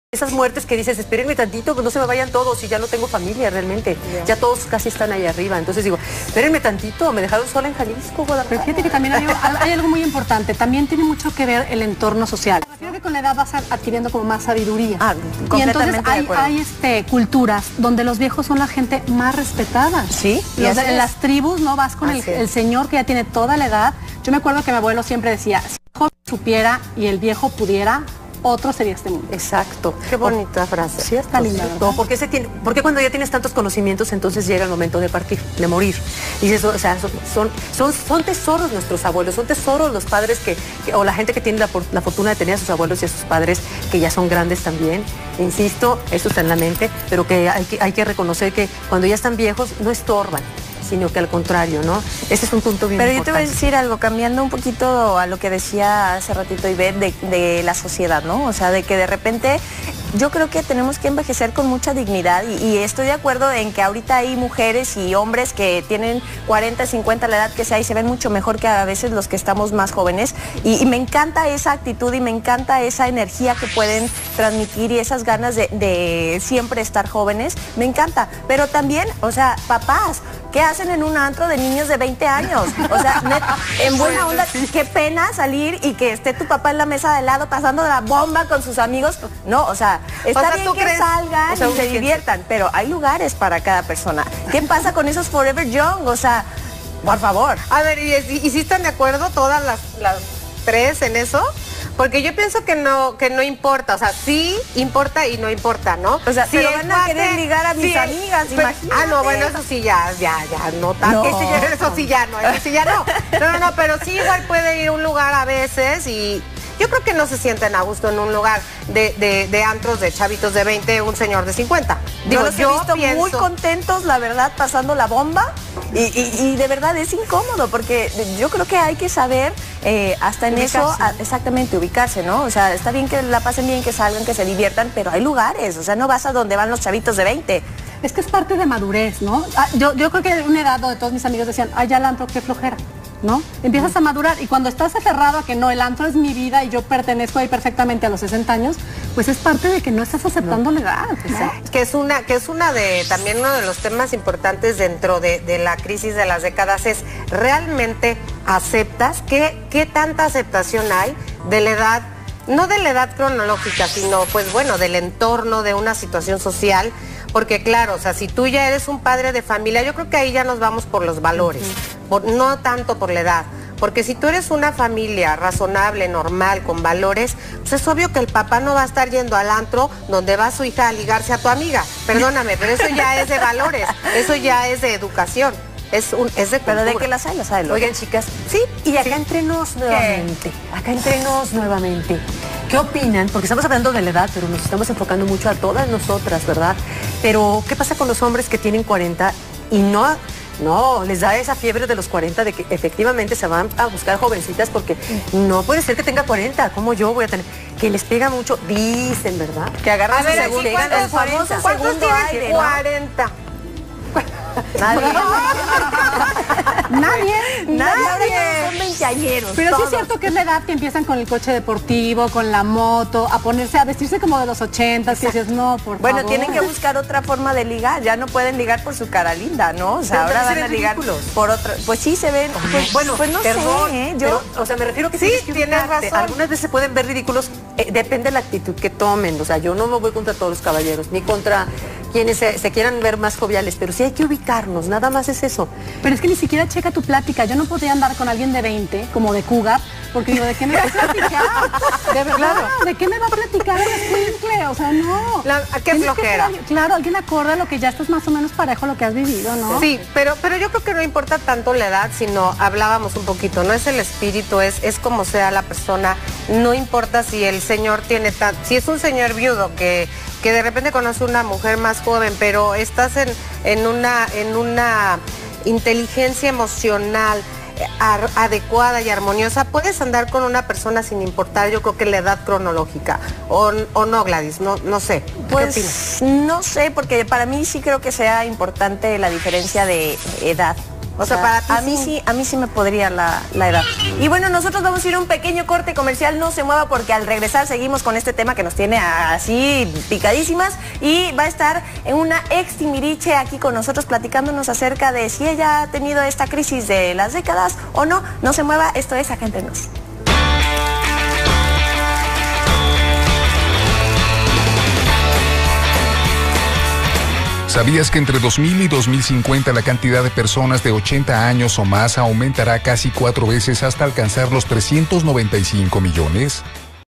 Acá entre Nos..La crisis de las décadas Parte 2 Tessy Arias
Continuamos platicando Acá entre Nos...nuestro tema sobre la mesa.."La Crisis de las décadas "